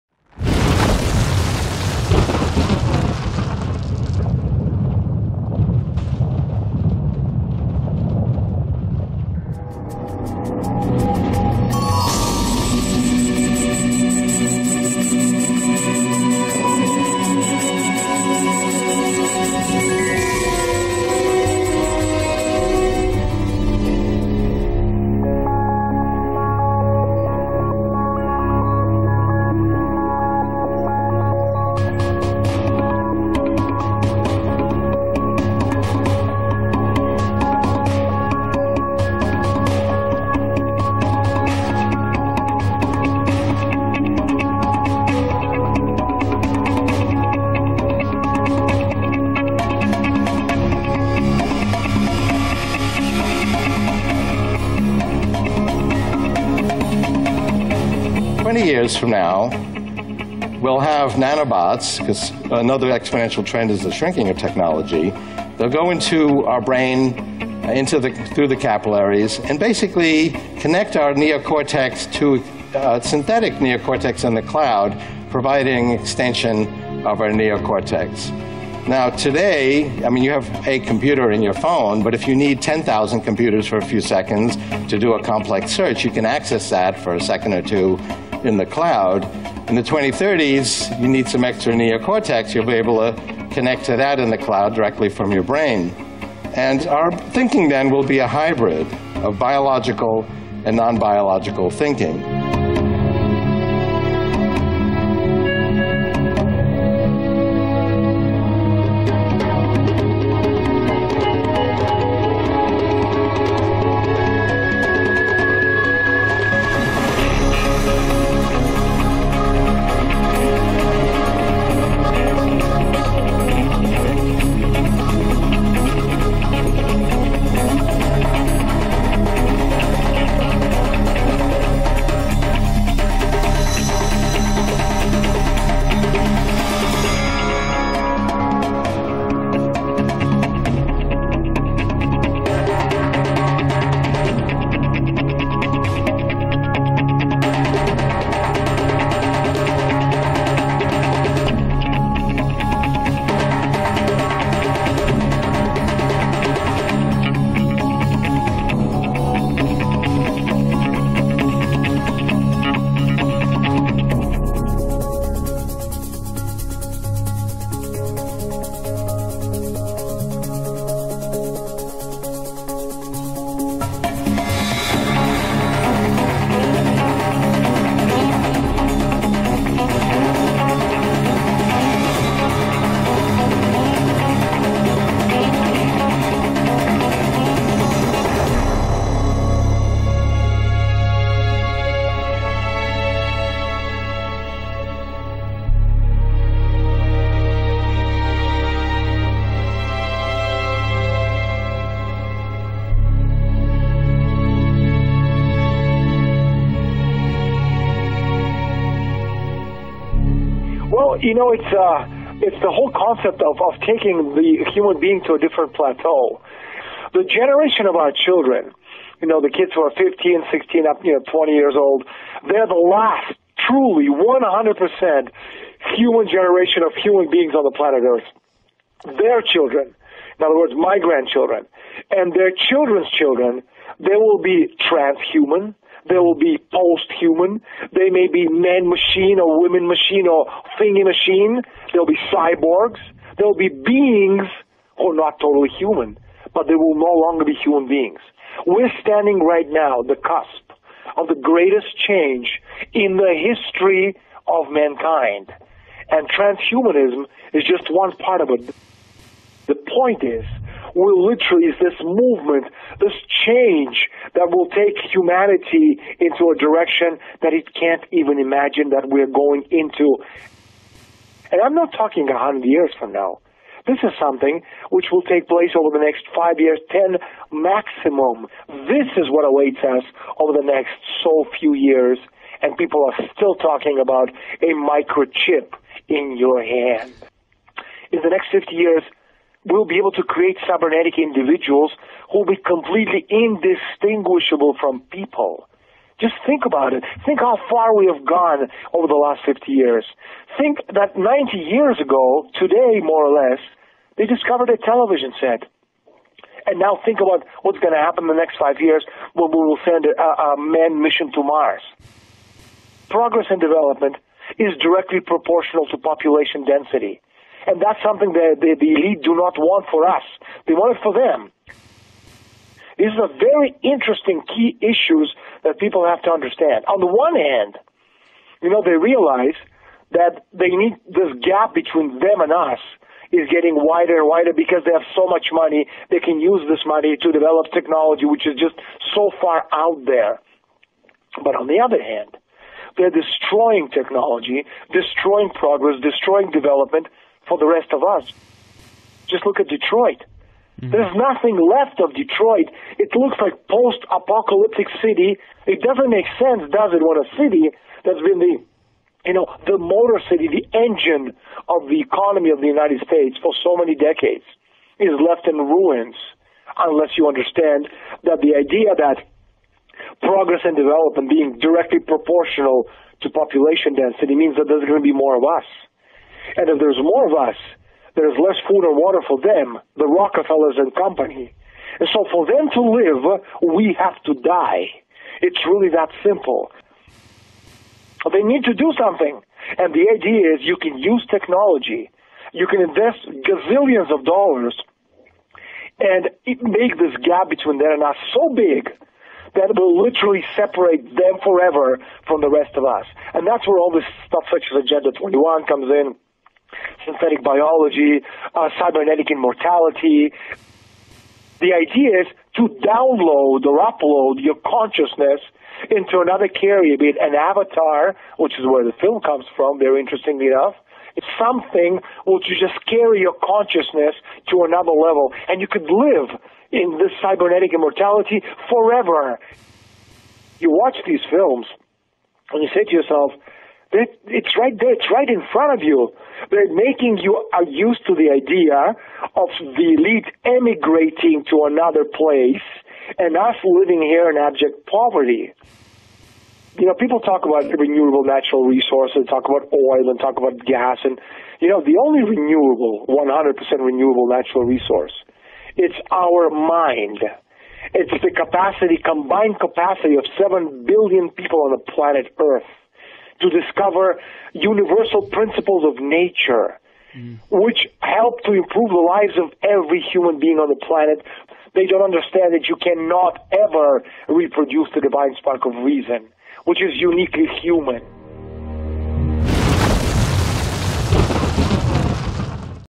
Transhumanismus - Damit sie leben können, müssen wir sterben
Google’s finsterer DNA-Plan
Transhumanismus - Neue Eugenik - Damit sie leben können, müssen wir sterben
Quelle und Beschreibung:
Ray Kurzweil von Google sagt: Die Menschen werden im Jahr 2030 "hybride", mit der Cloud verbundene Gehirne haben.
In 20 Jahren werden wir Nanoroboter haben. Sie werden in das Gehirn eindringen, über die Kapillargefäße, dann mit einer künstlichen Gehirnrinde in der Cloud verbunden, wodurch unsere Gehirnrinde erweitert wird.
Daniel Estulin: Es handelt sich um das Konzept, den Menschen auf eine andere Ebene zu bringen.
Unsere heutige Generation, Kinder und Jugendliche, ist die letzte Generation die wirklich zu 100% menschliche Wesen sind.